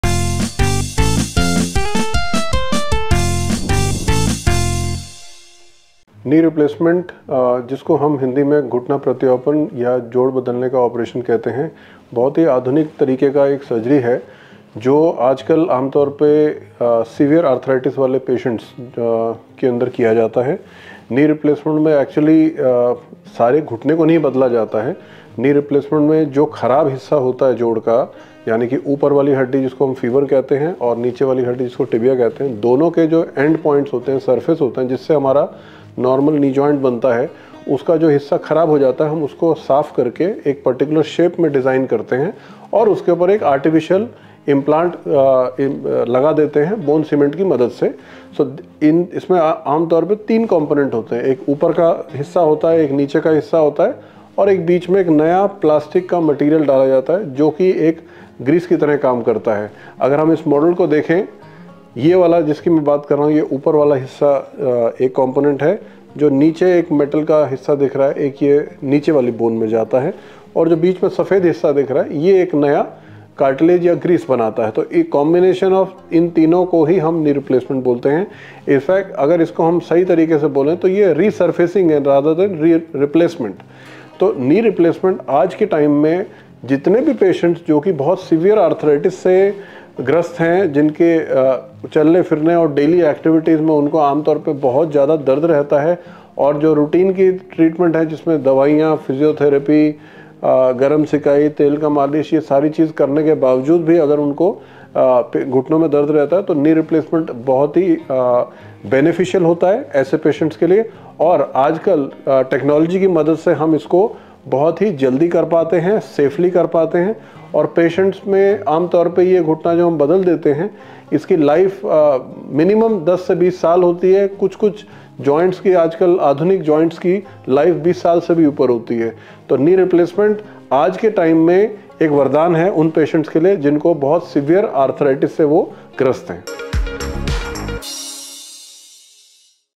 Knee Replacement Surgery Procedure: जानें कैसे होता है घुटने का ऑपरेशन, कितना लगता है टाइम?
अगर आप Knee Replacement रिप्लेसमेंट करने जा रहे हैं या इससे जुड़ी जानकारी प्राप्त करना चाहते हैं, तो वीडियो में डॉक्टर इस प्रक्रिया से जुड़ी सारी जानकारी आपको देंगे।